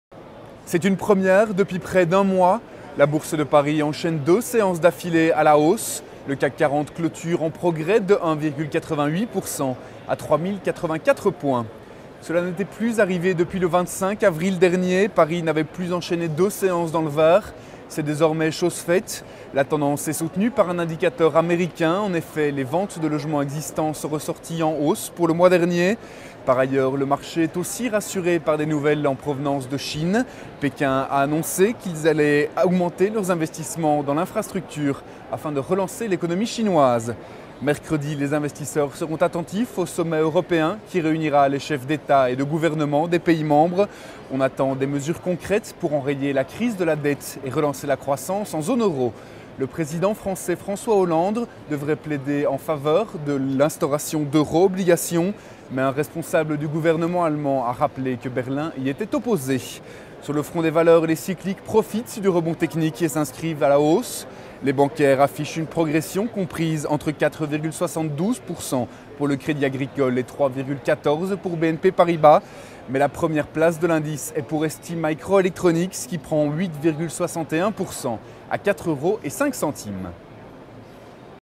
Paris enchaine une deuxième séance en hausse
Clôture Europe 22/05/2012 Pour la première fois depuis près d'un mois, la Bourse de Paris a enchainé une deuxième séance dans le vert, grâce à un indicateur américain et de bonnes nouvelles chinoises. A la clôture, le CAC prend 1.88% à 3084 points.